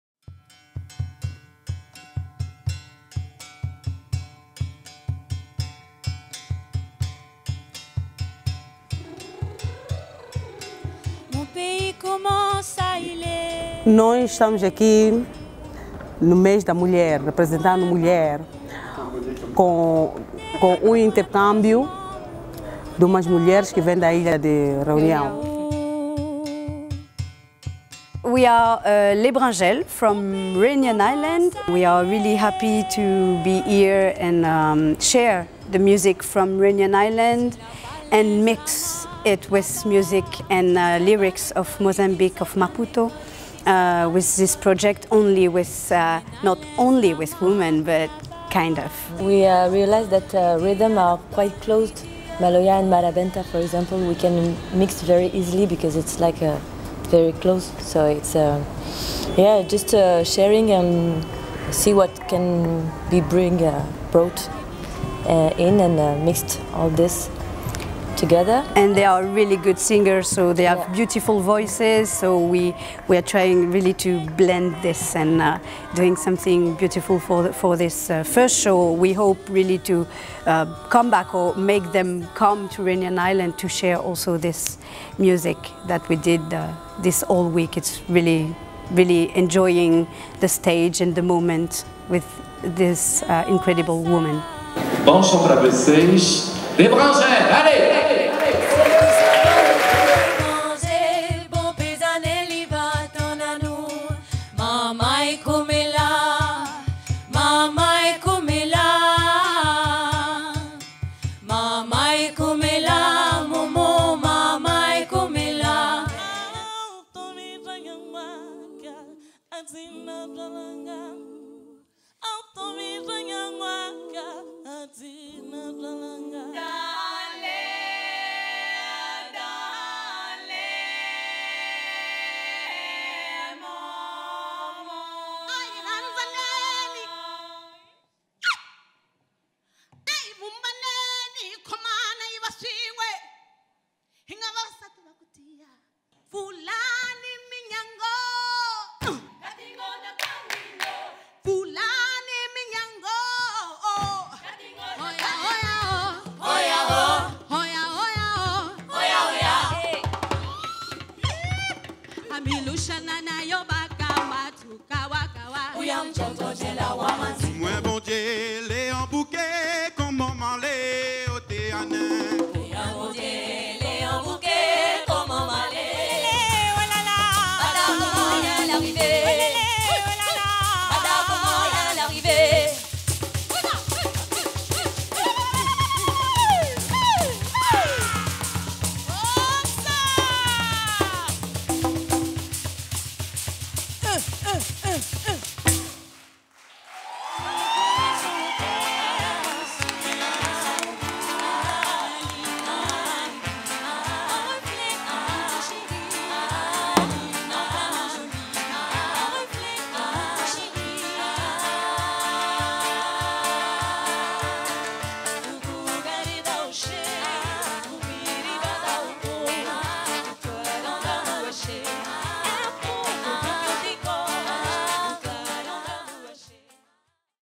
Les Bringelles em Moçambique
Acompanhamos esta residencia artistica e intercambio entre artistas da Ilha de Runião e artistas de Moçambique, este intercambio terminou com um show no Centro Cultural Franco-Moçambicano